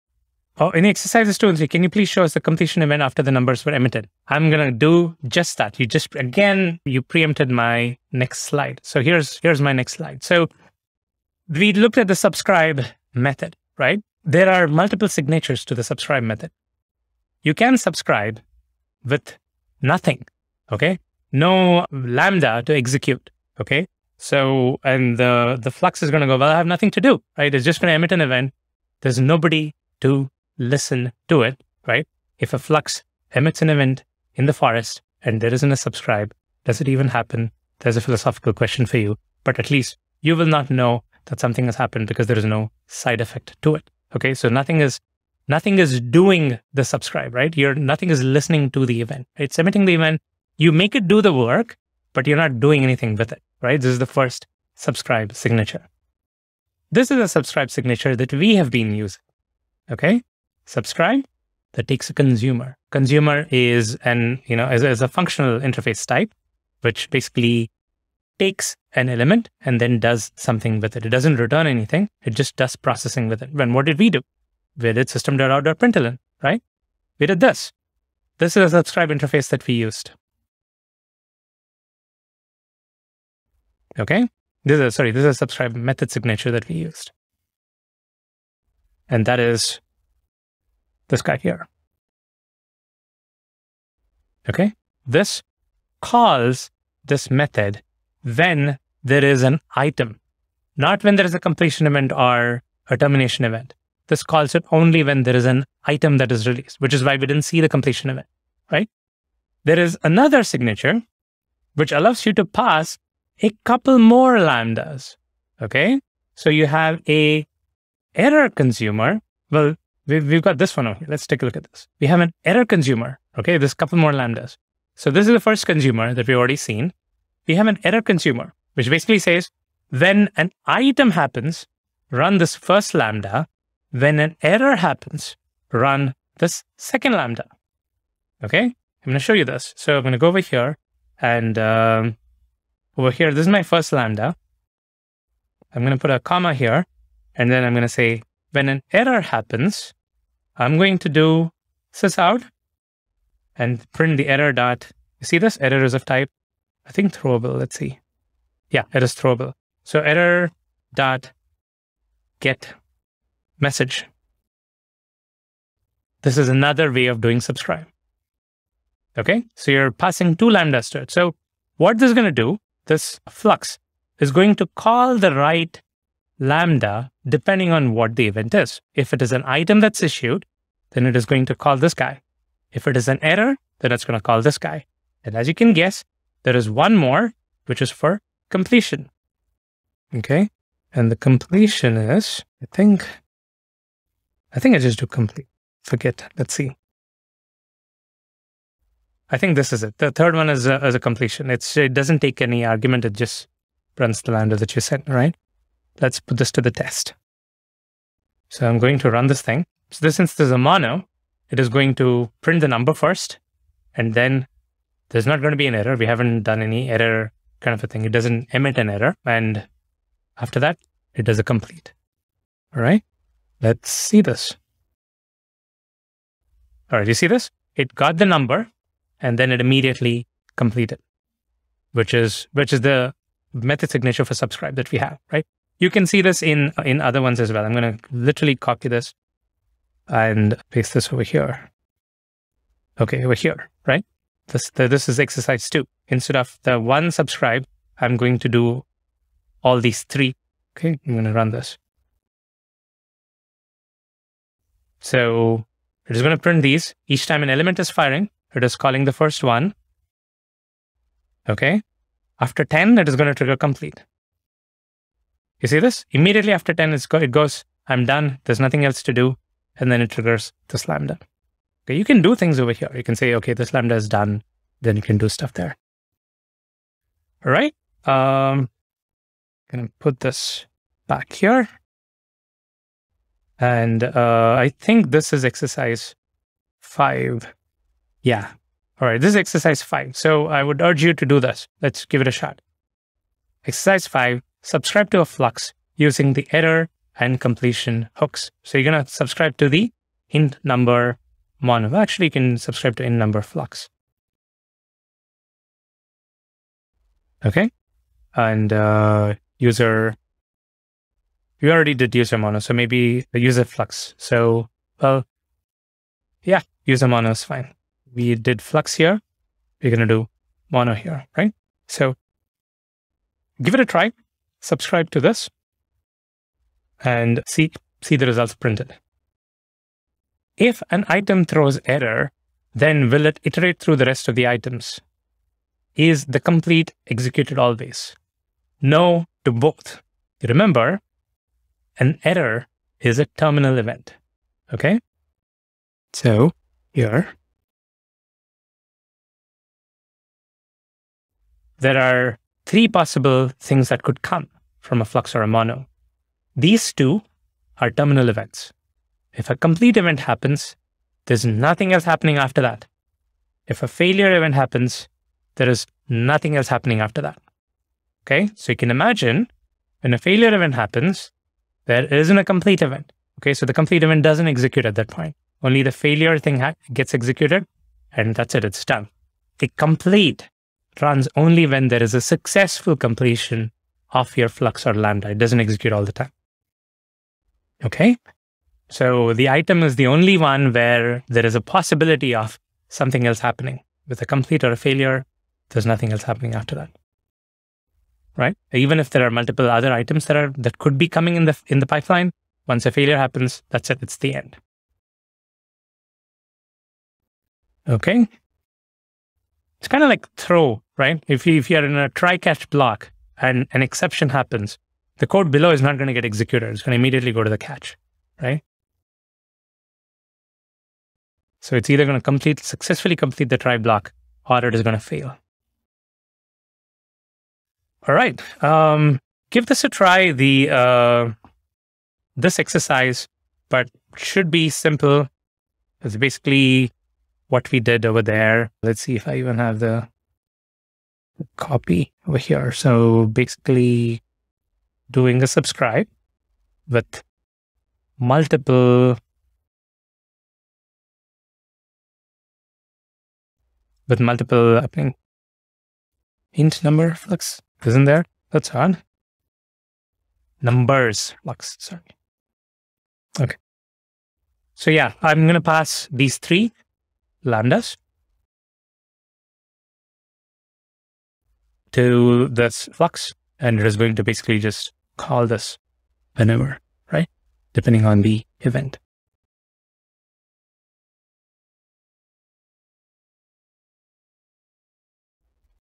18 The subscribe method (Reactive programming with Java - full course)
Course: Reactive programming in Java
Covers: Reactive fundamentals, Project Reactor

Access this full course NOW & unlock more awesome courses like this by becoming a member:

Learn the basics of reactive programming. You will learn to "THINK REACTIVE" - understand the paradigm shift and thinking change necessary to write code in a reactive way using Project Reactor.